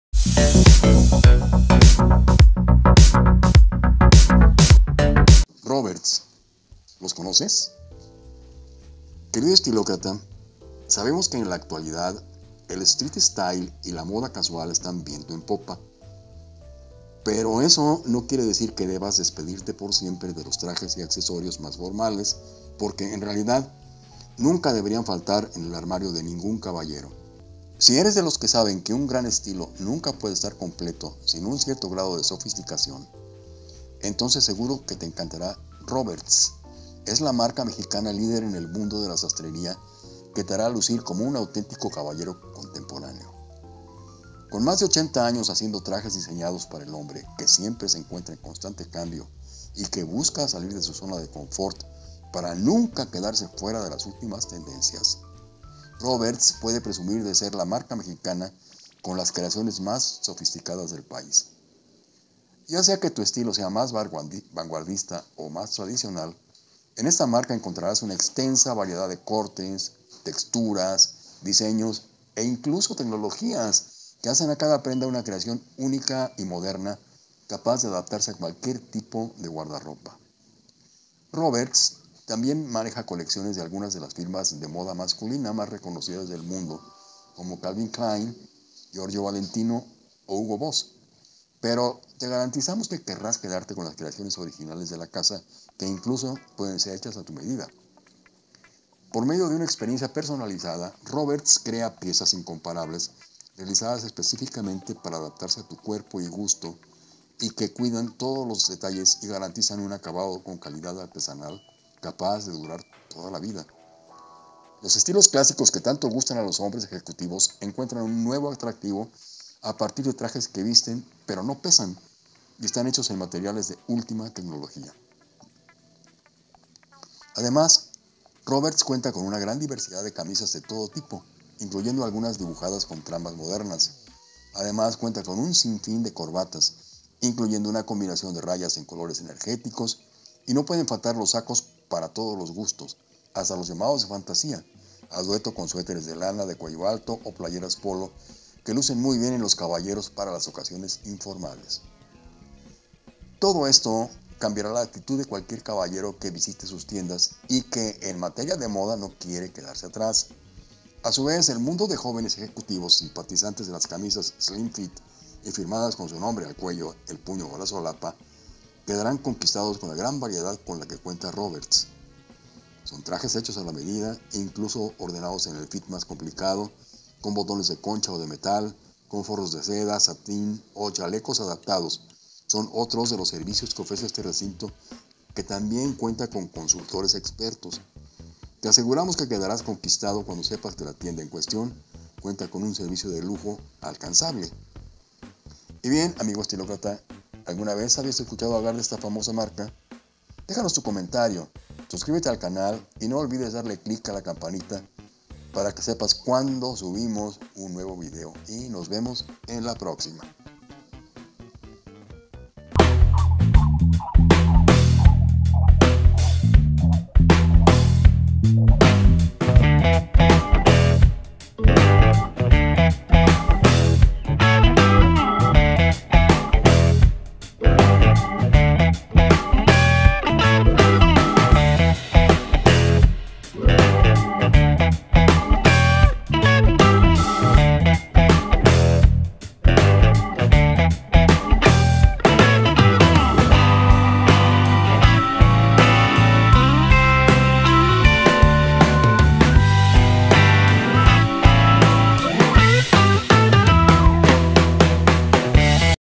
☛ TRAJES ROBERTS ¿SON BUENOS O MALOS? 🕵🏼‍♀️
Puedes aquí revisar si estos trajes son lo tuyo y un vistazo sobre qué modelos trae

Visita nuestro sitio asociado de Amazon para productos de tu interés: